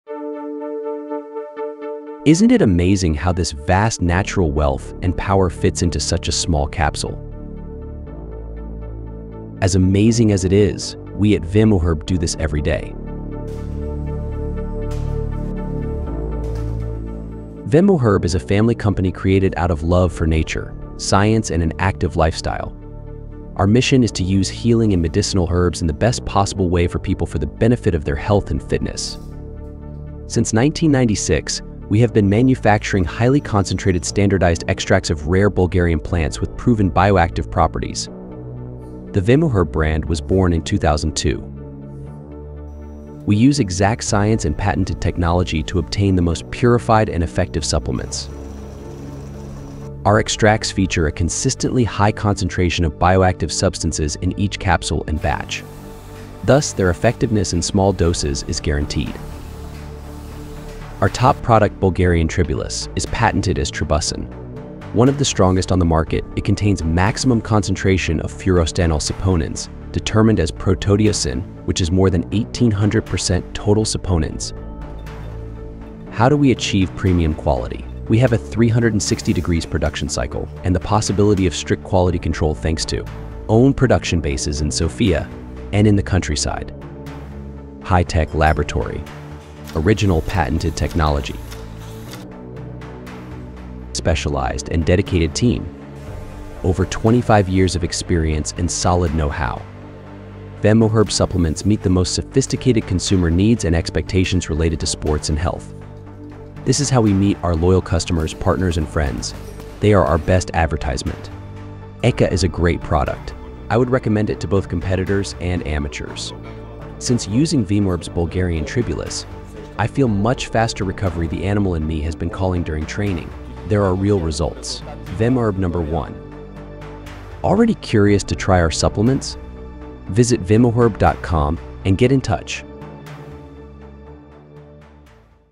We are the leading manufacturer of Bulgarian Tribulus patented as Tribuscin
VemoHerb's recipe for manufacturing really effective, bioactive sport & health supplements includes 360-degree production cycle and strict quality control at each step of the process. The manufacturing of our bioactive herbal extracts is supported by:

High-tech production facilities;
Specialized scientific team;
Patented technology;
In-house laboratory;
Solid know-how developed and refined over 25 year of experience.